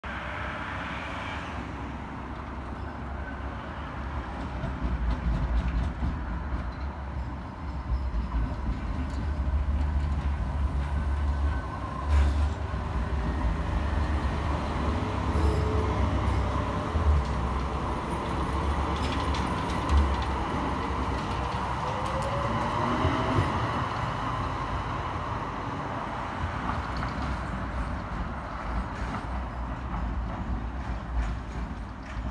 Sheffield Supertram 125 arrives into Middlewood with a Yellow Route Service from Meadowhall
Stagecoach Supertram 125 seen arriving into Middlewood while working an Yellow Route Service from Meadowhall Interchange on Tuesday 23 of August 2022